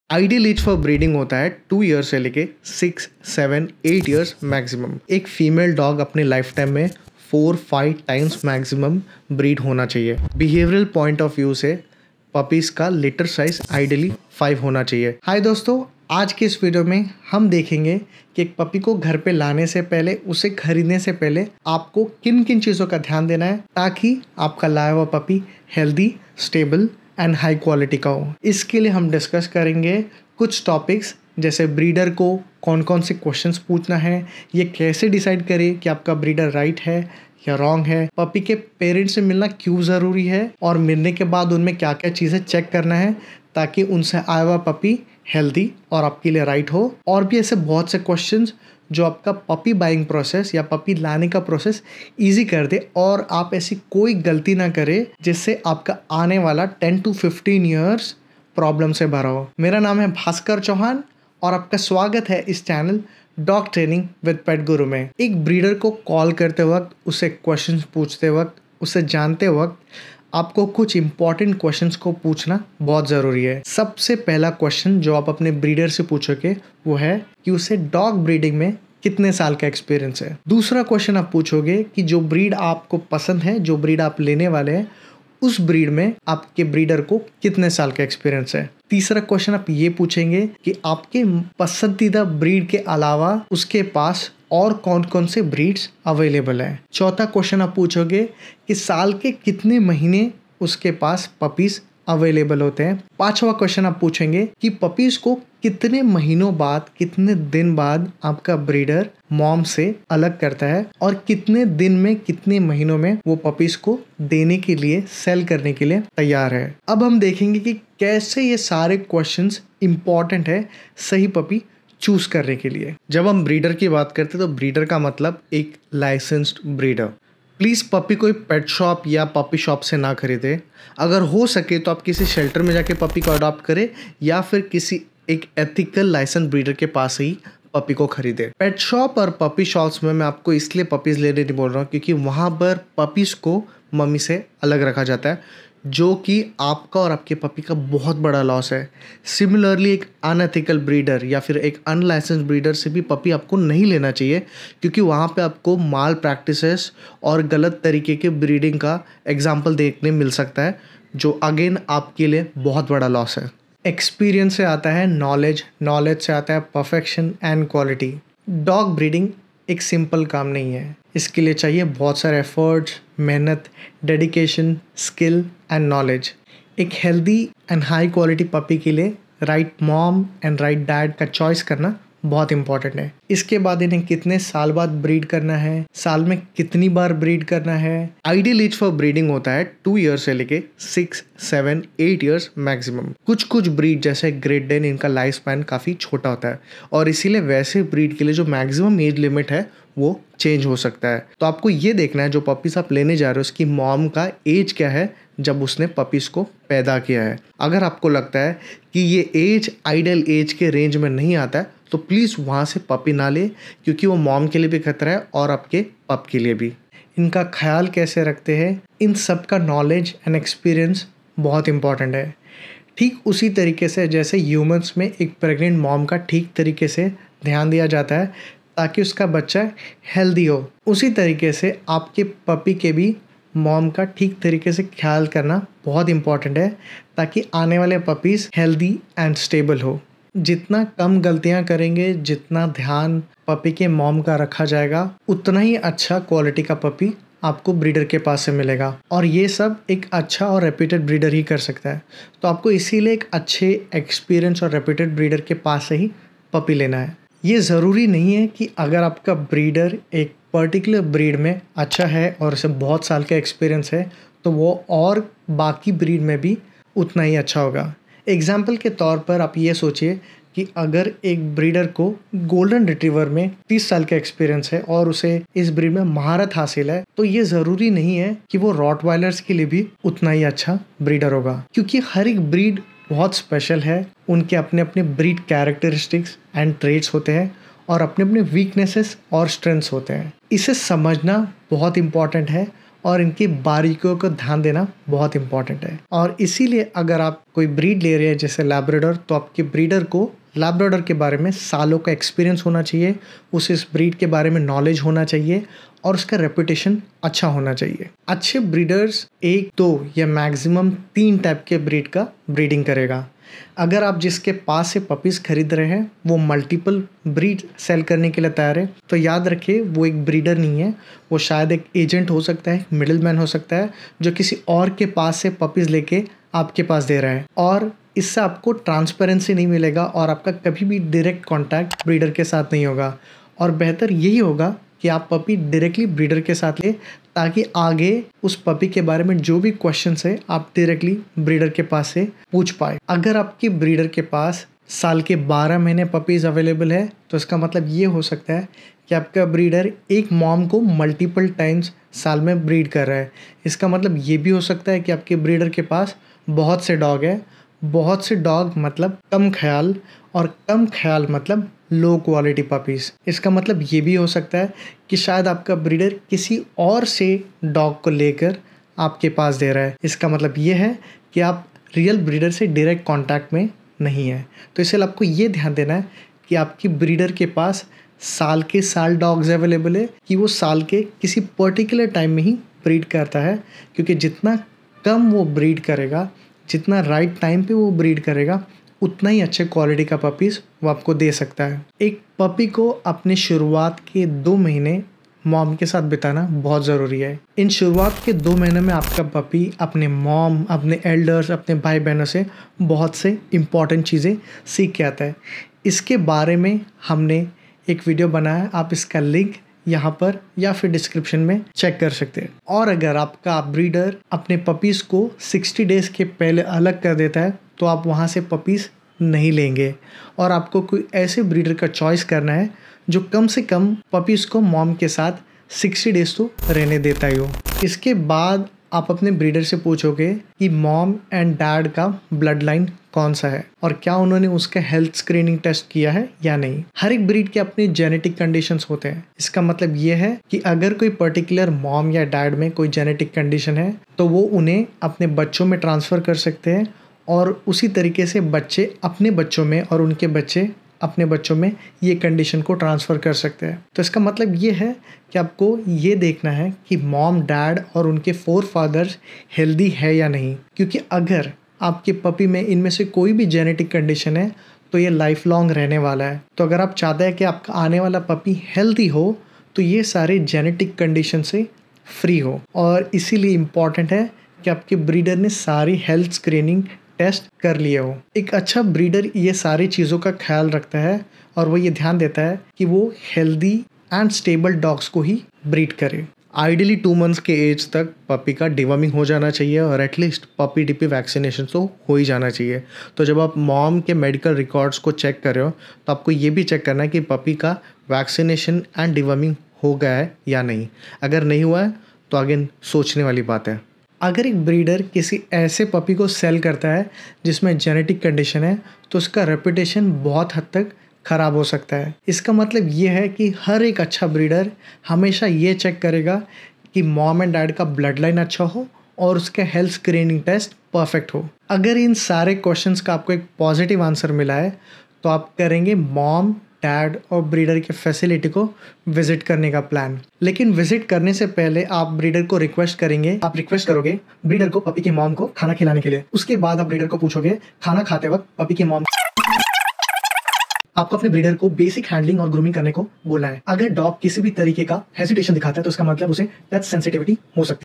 The most important questions to ask your dog breeder before buying a puppy part 1
The most important question to ask your dog breeder is a very important video as it will tell which important questions to ask your dog breeder before buying a dog. Dog breeders may or may not tell answers to all the questions and you need to ask the above questions. dog breeding buying a puppy